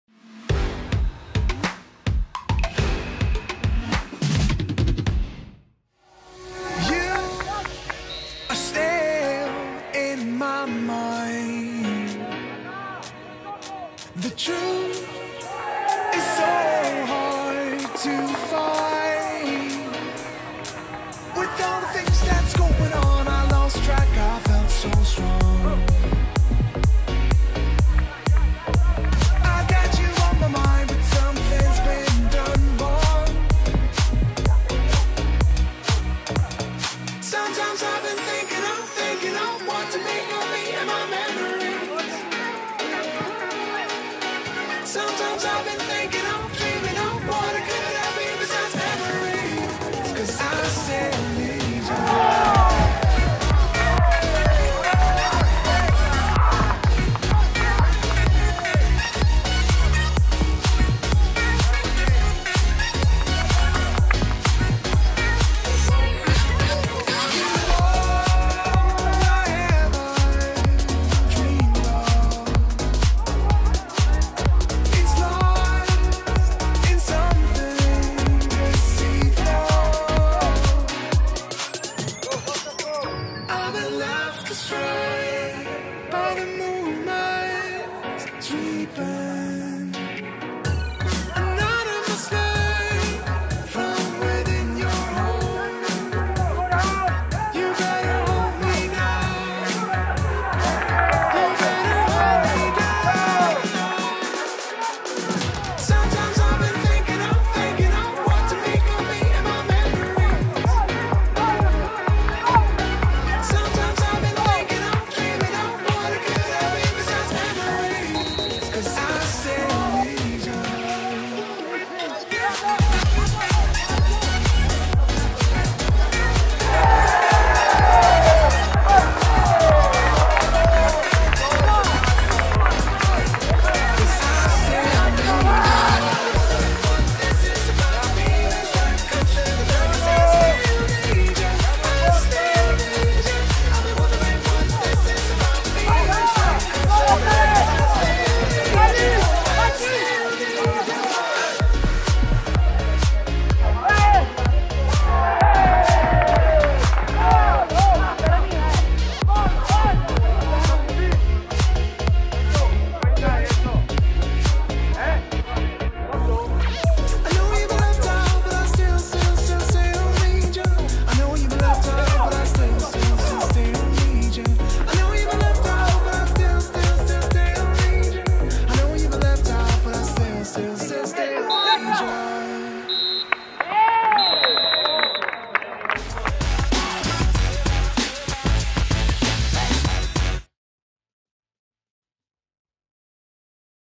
Highlights Primavera 1 TIM: Lazio-Sampdoria 1-4
Primavera 1 TIM, 17.a giornata. I gol e le azioni salienti del confronto tra i i biancocelesti e i blucerchiati di Tufano.